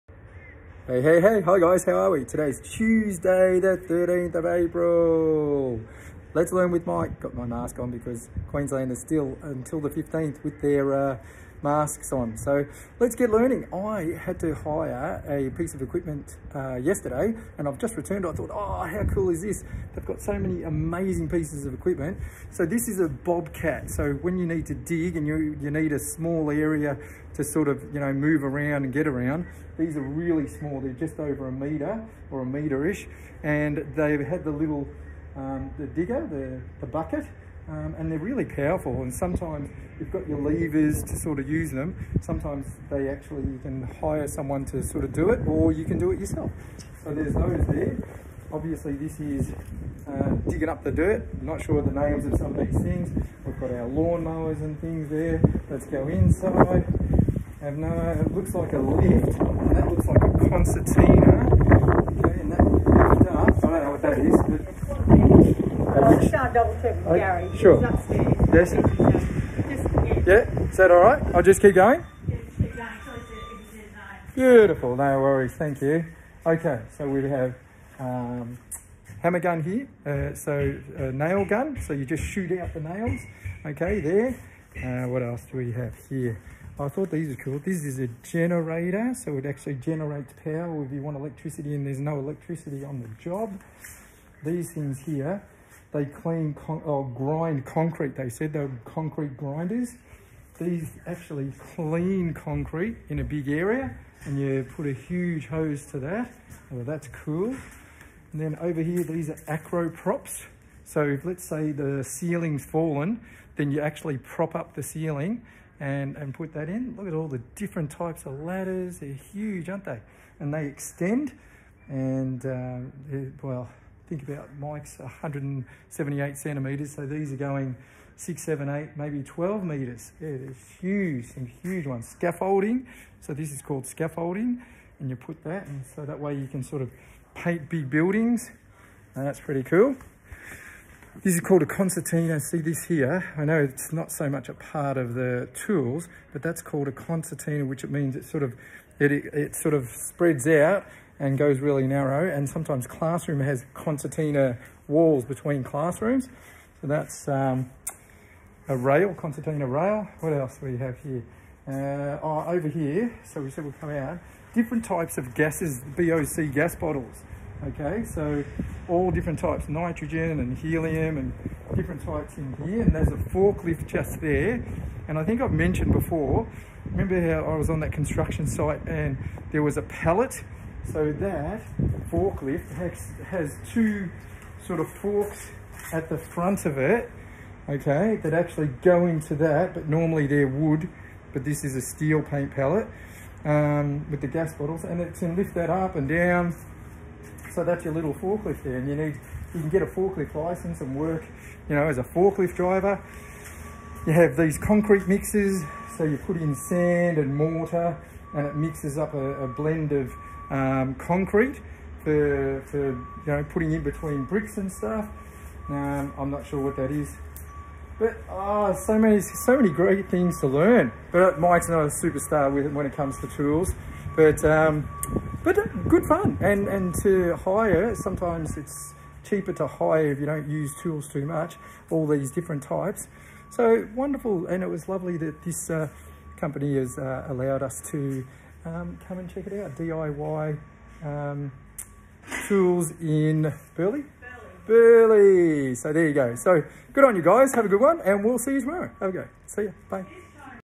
Let’s Learn with Mike - April 13th 2021
Today I am at DIY (Do It Yourself) tool hire and I discuss many different tools (not than I’m an expert!).